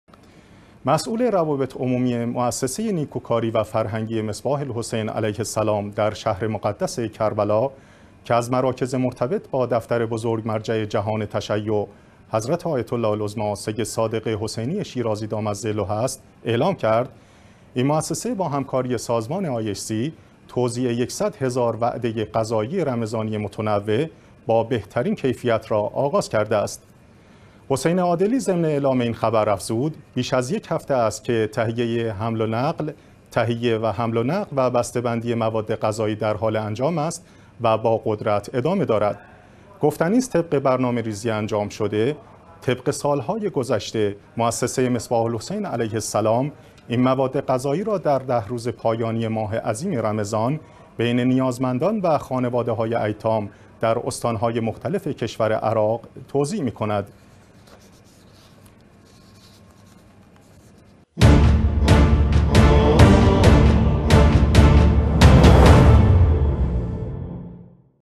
آغاز توزیع ۱۰۰ هزار وعده غذایی رمضانى در عراق توسط موسسه مصباح الحسین علیه السلام و سازمان IHC
مسئول روابط عمومى موسسه نيکوکارى و فرهنگى مصباح الحسین علیه السلام در شهر مقدس کربلا، که از مراکز مرتبط با دفتر بزرگ مرجع جهان تشیع، حضرت آیت الله العظمی سید صادق حسینی شیرازی، دام ظله است، اعلام کرد این موسسه با همکاری سازمان IHC ، توزیع ١٠٠ هزار وعده غذایى رمضانى متنوع، با بهترین کیفیت را آغاز کرده است.
حسین عادلى ضمن اعلام این خبر، افزود: بیش از یک هفته است که تهیه، حمل و نقل و بسته بندى مواد غذایی در حال انجام است و با قدرت ادامه دارد.
گفتنی است، طبق برنامه ریزى انجام شده، طبق سال‌های گذشته، موسسه مصباح الحسین علیه السلام این مواد غذایى را در 10 روز پایانی ماه عظیم رمضان بین نيازمندان و خانواده‌هاى ایتام در استان‌های مختلف کشور عراق توزیع مى‌کند.